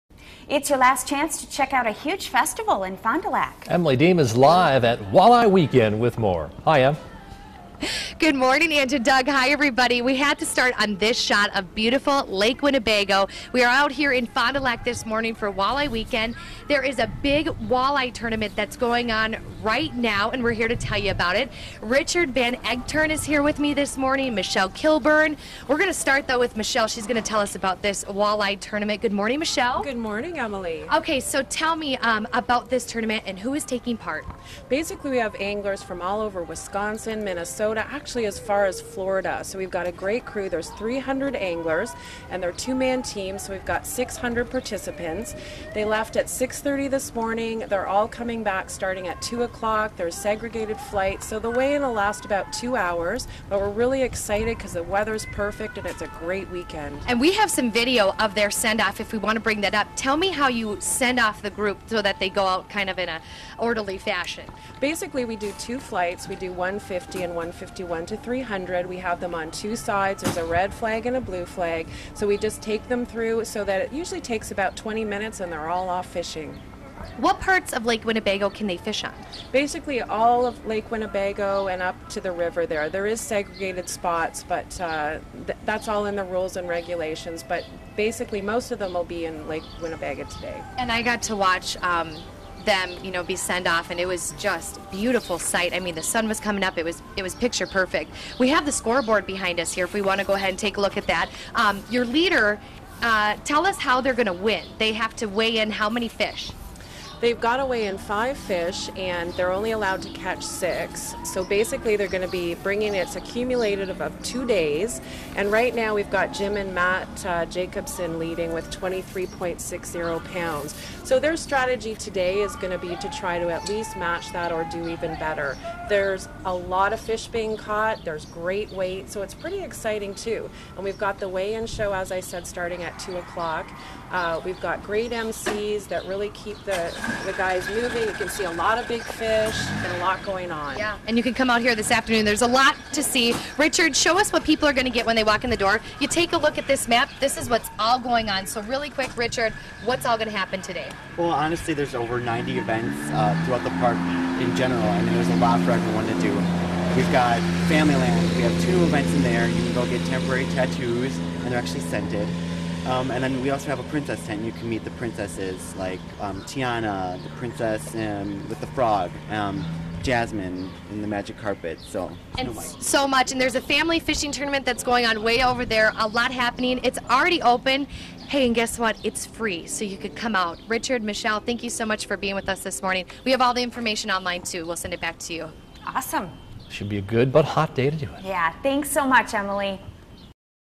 GDW8SUN Walleye Weekend
Michelle Kilburn talks about the walleye fishing tournament at Walleye Weekend.